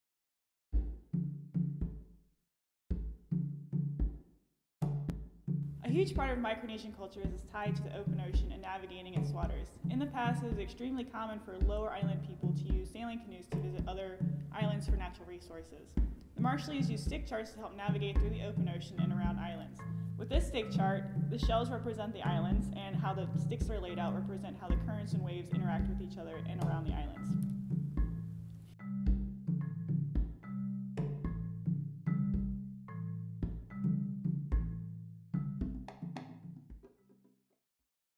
Exhibit: Arts and Culture of Micronesia
A huge part of Micronesian culture is its ties to the open ocean. Kelsey Hansen, Group Tours and Education Coordinator at MOAS shows us an example of a stick chart created by the Marshallese to navigate the ocean. To learn more, come explore the exhibit: Arts and Culture of Micronesia.

An exhibit showcasing many cultural and artistic relics from Micronesia. Arts and Culture of Micronesia featuring many diverse objects and traditional crafts from the personal collection of Barbara Wavell.  Model-size canoes used for open ocean voyages, traditional baskets and bowls, storyboards depicting ancient tales, and numerous objects unique to these tiny islands will be on display through late February 16, 2020.